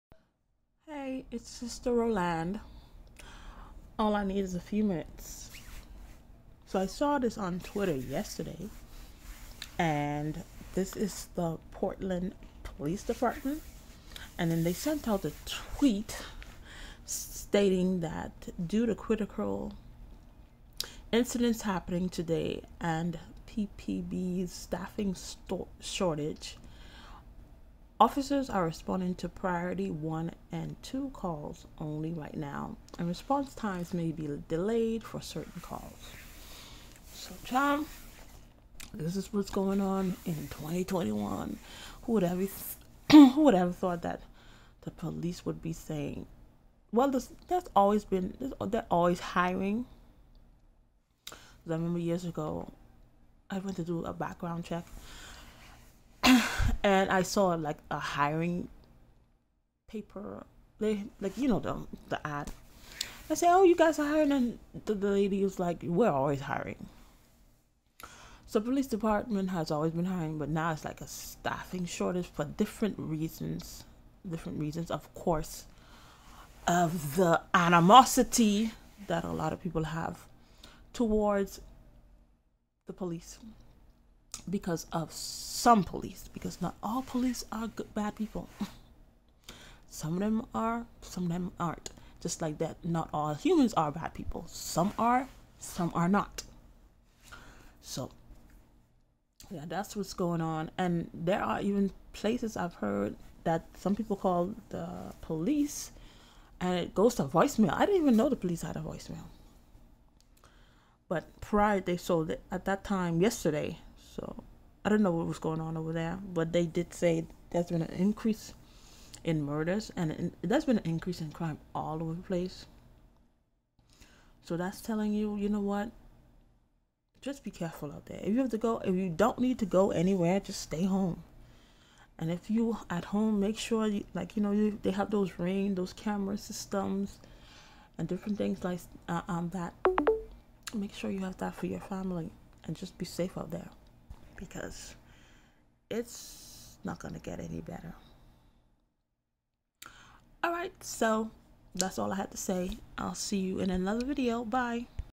Portland Police Is Short Staffed/ Trying To Combat Crime
The Portland Police Department tweeted on December 6th 2021, they were only able to respond to Priority 1 & 2 calls, all other calls may experience delayed response times. All I need is a few minutes to discuss this.

Instagram: Rolande Mary St Preux (@loveroffood.rolande) • Instagram photos and videos